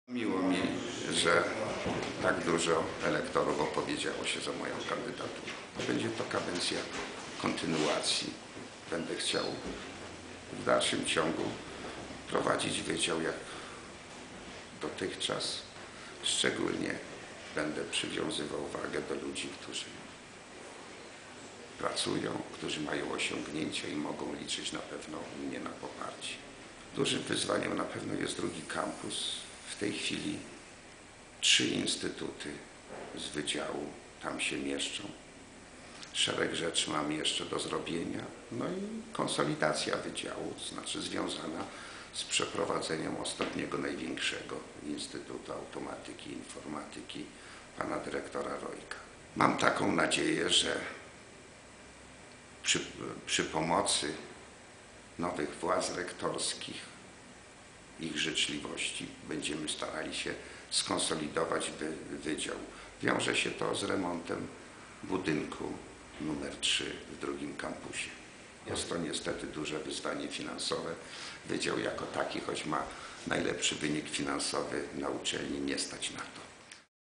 Dziekan elekt WEAiI - prof. dr hab. inż. Marian Łukaniszyn
Prof. dr hab. inż. Marian Łukaniszyn został wybrany na dziekana Wydziału Elektrotechniki, Automatyki i Informatyki w kadencji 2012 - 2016.